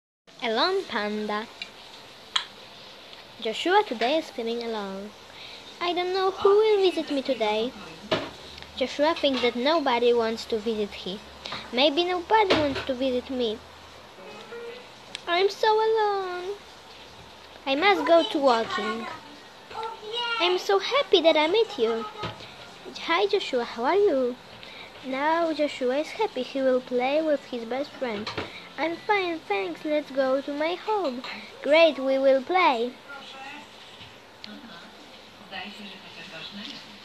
Anitales: "Alone Panda " - ijja
'Joshua today is feeling alone.'. 'Alone Panda ' created by ijja using the Anitales app.